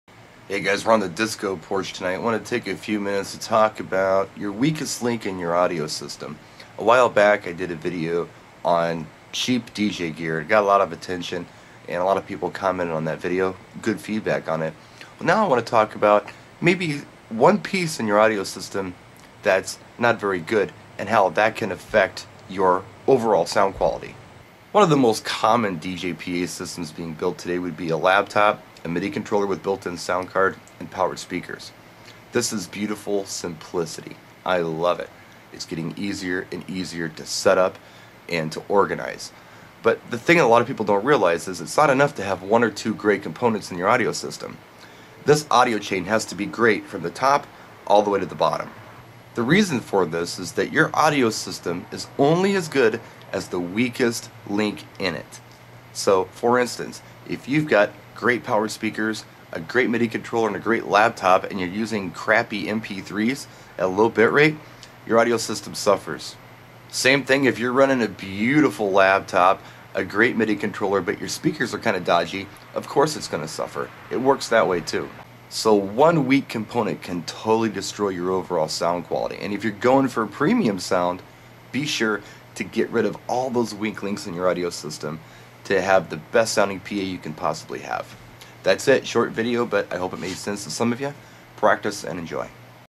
Cheap DJ Gear - Audio's Weakest Link
Quick video expressing the importance of consistant quality all the way thru your "Audio Chain"